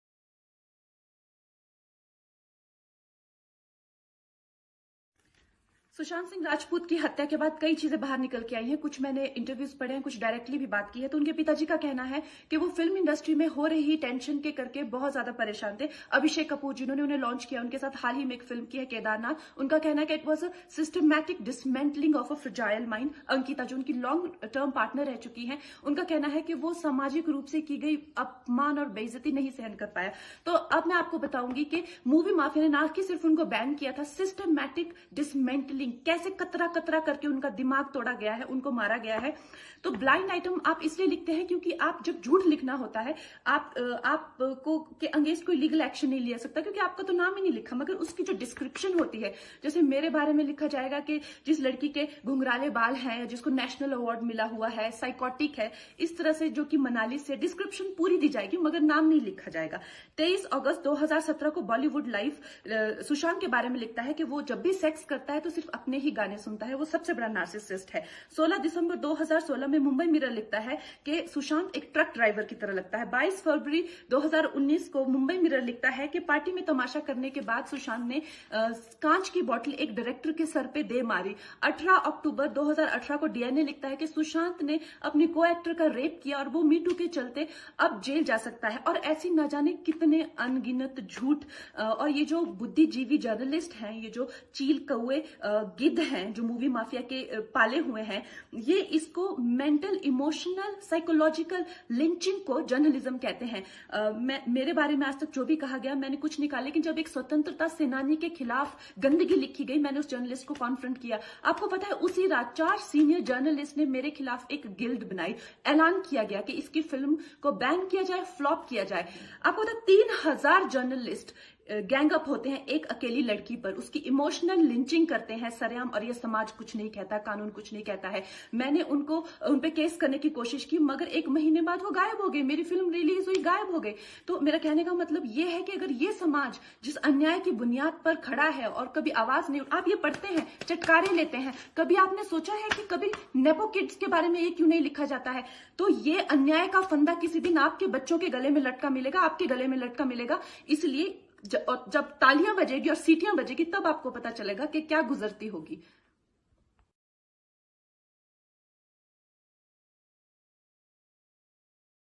kangana Ranaut ने किया बड़ा खुलाशा Bollywood का हुआ नाम बदनाम
Kangana Ranaut New Video On SUSHANT SINGH RAJPUT $suicid
e

Exposed Bollywood Nepotism

Kangana Ranaut ने किया बडा  खुलासा
Bollywood का हुआ नाम बदनाम

For new trending videos.

..Only treanding..